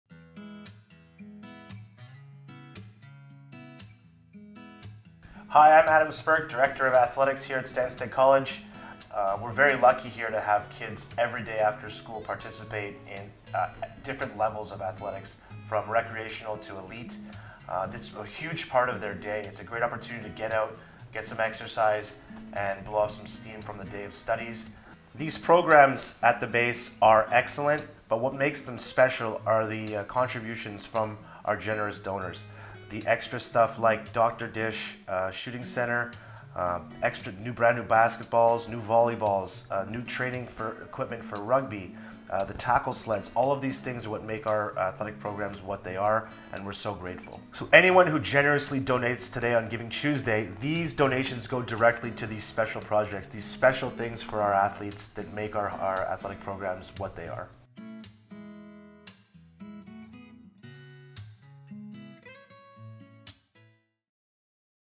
GivingTuesday: Athletics
Director of Athletics, Adam Spirk, talks about the benefits of the sports program and how donations can help with every athletes experience.